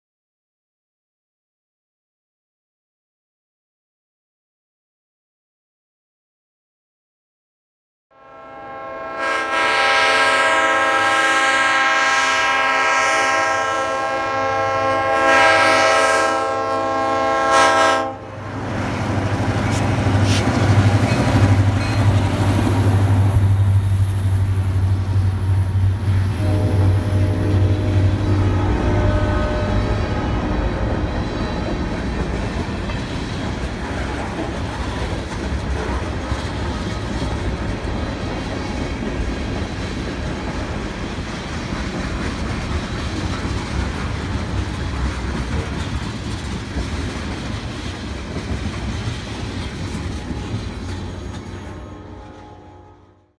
Buffalo & Pittsburgh RR WAER
The Buffalo & Pittsburgh Railroad WAER crossing McClelland Ave. in Erie, PA on 2/15/22.